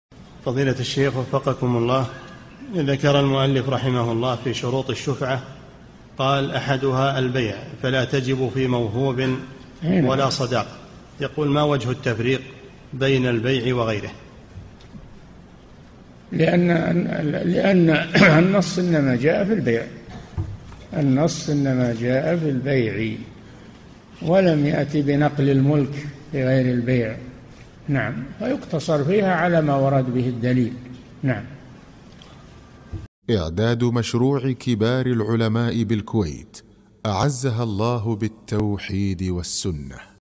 [1957 -3022] في قول المؤلف في شروط الشفعة: أحدها البيع، فلا تجب في موهوب ولا صداق، ما وجه التفريق..
-"[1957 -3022] السؤال: ذكر المؤلف رحمه الله في شروط الشفعة، قال أحدها البيع فلا تجب في موهوبٍ ولا صداق، يقول: ما وجه التفريق بين البيع وغيره؟
الجَوَابُ: لأن النص إنما جاء في البيع، النص إنما جاء في البيع، ولم يأت بنقل الملك بغير البيع. فيقتصر فيها على ما ورد به الدليل.
للاشتراك في قناة الشيخ صالح الفوزان

للاستفسار والإقتراحات والدعم
رئيس الفريق
لمشروع كبار العلماء